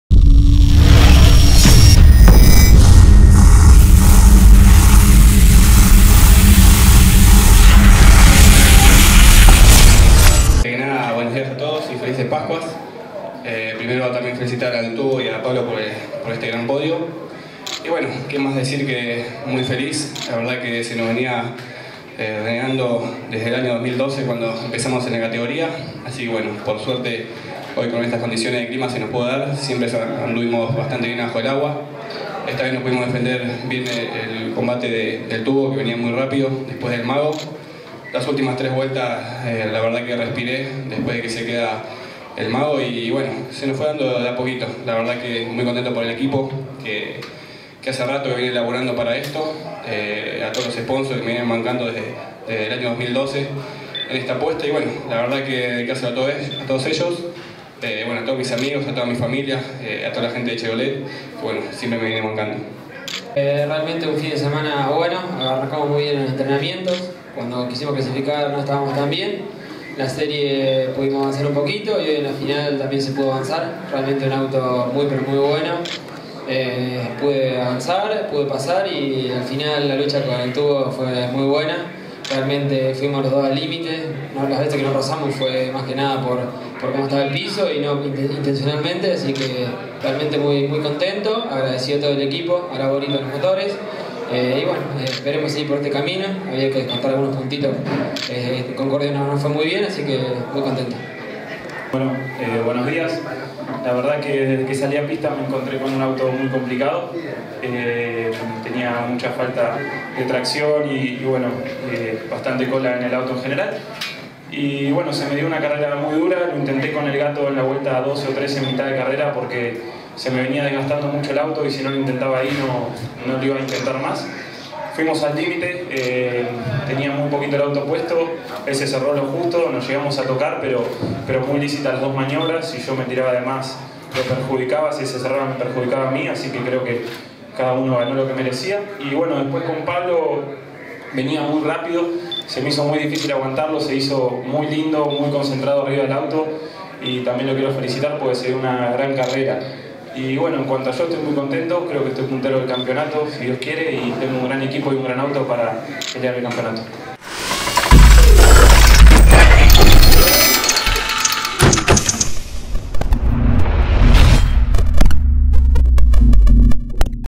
Final del Turismo Carretera Pista.
Gaston Crusitta, Pablo Constanzo y Esteban Gini.